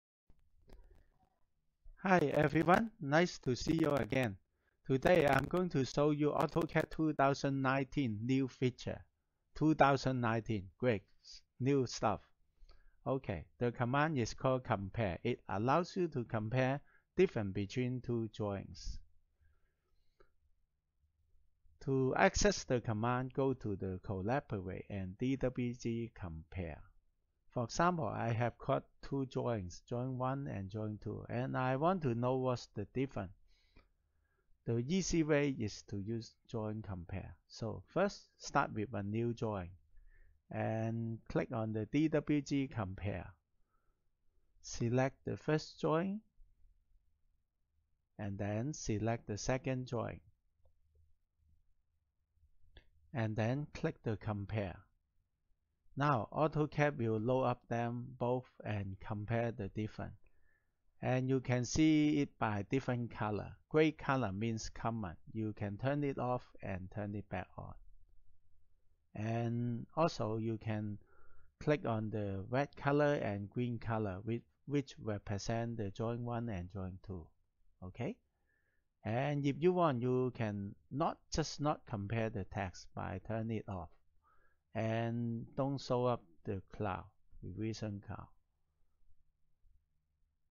AutoCAD 2019 New Features - Compare command
Compare command allows you to compare the different betwen drawings easily.